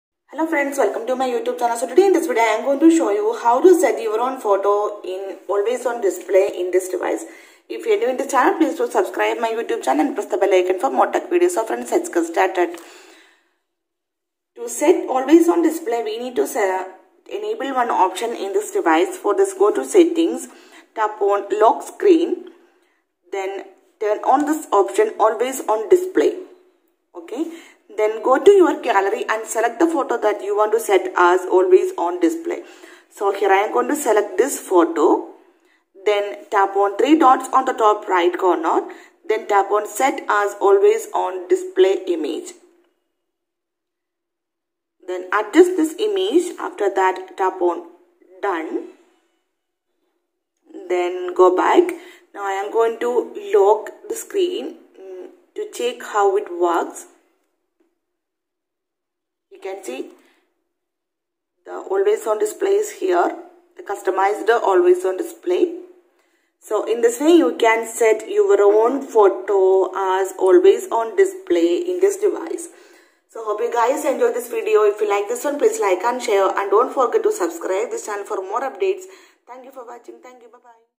Samsung Galaxy A12 | Customize AOD in Samsung Galaxy A12
HI friends this video shows you that " How to set/custome AOD in samsung galaxy a12 "